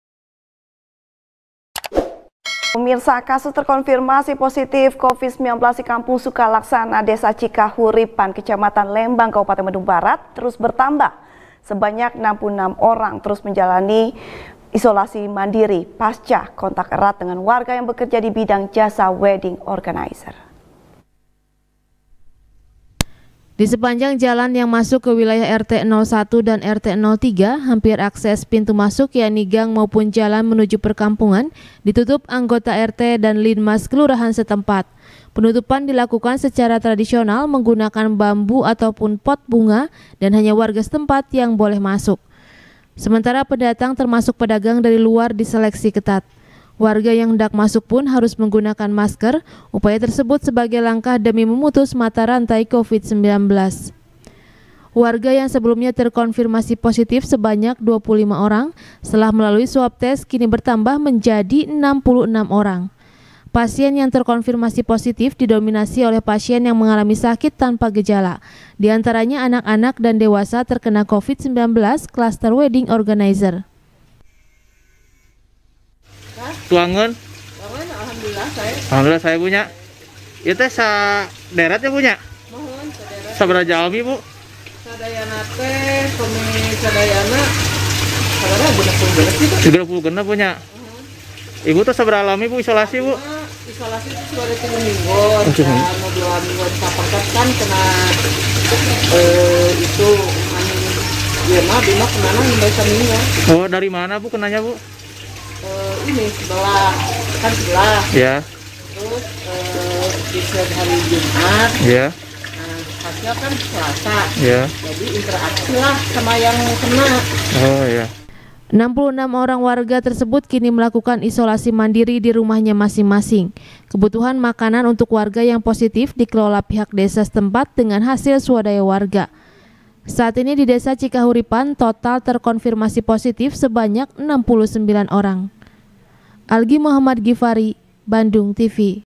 66 Orang, Kasus Positif Klaster Wedding Organizer | SBR | BANDUNGTV
SBR | BANDUNGTV | FARHAT MUMTAZ | Kasus terkonfirmasi positif Covid 19, di Kampung Sukalaksana, Desa Cikahuripan,Kecamatan Lembang,Kabupaten Bandung Barat, terus bertambah. Sebanyak 66 warga harus menjalani isolasi mandiri, pasca kontak erat dengan warga, yang bekerja di bidang jasa wedding organizer.